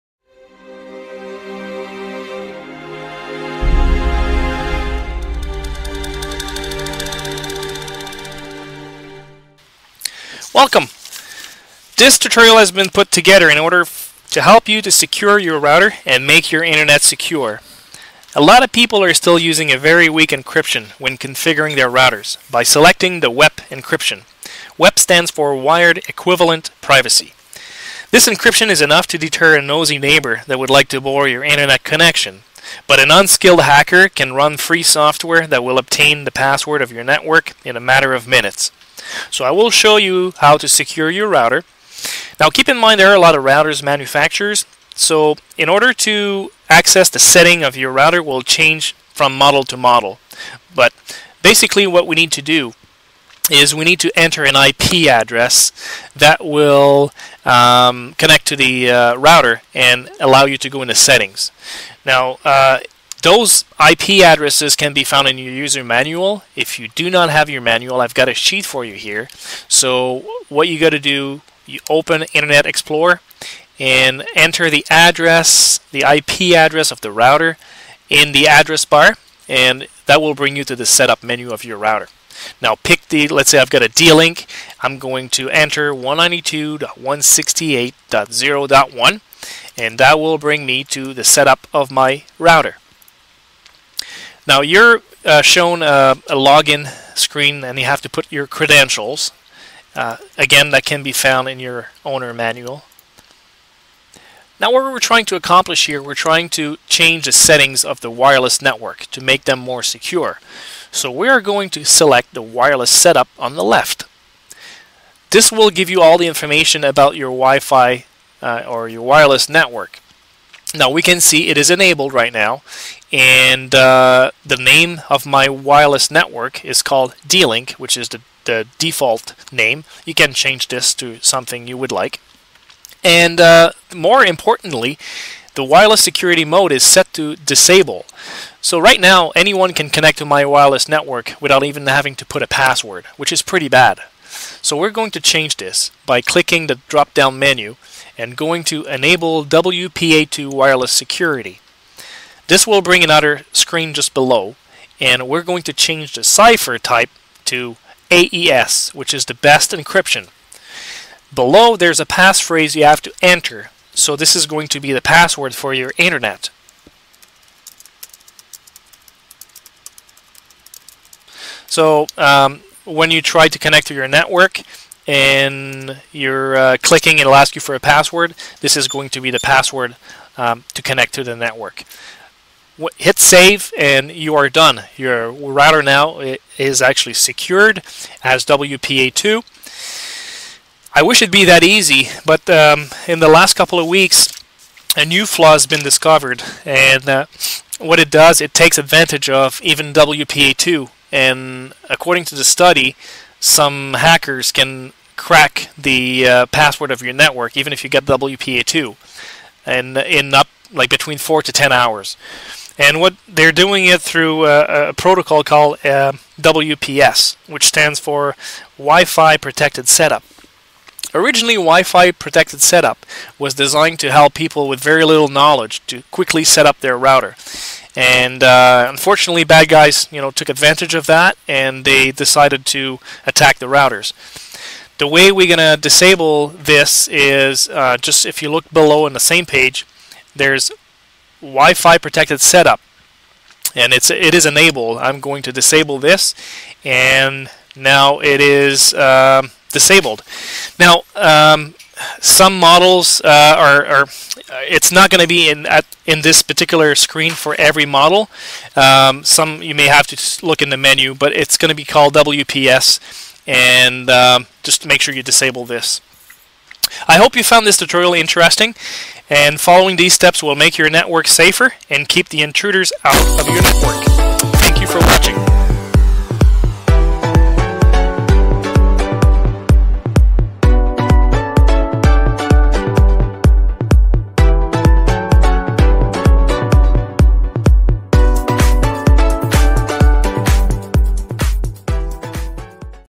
How to easily Secure your Home WiFi Network
Network security tutorial that will teach you how you can easily secure your Wi-Fi, enhance the security of your router, firmware and network overall and How to secure your wifi against attacks